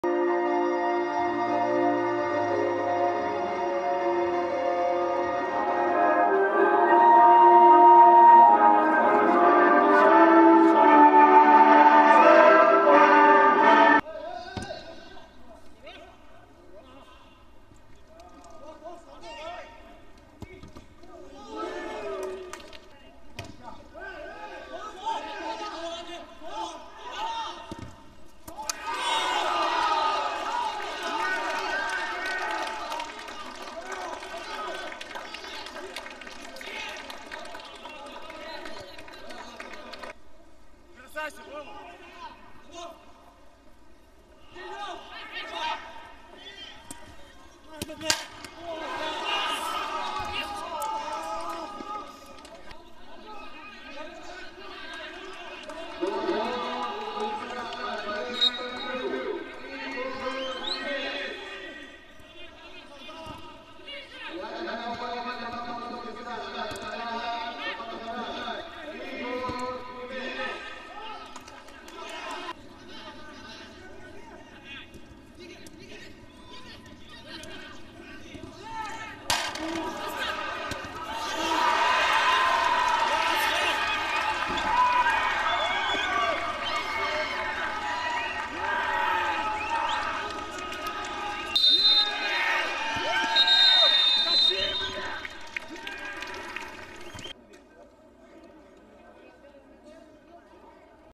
Uzbekistan vs China PR: AFC U22 Championship 2014
Uzbekistan 2-1 China: Egor Krimets and Igor Sergeev scored in stoppage time as Uzbekistan claimed a remarkable 2-1 comeback win over China in Sunday's AFC U-22 Championship Group D opener at Seeb Sports Complex.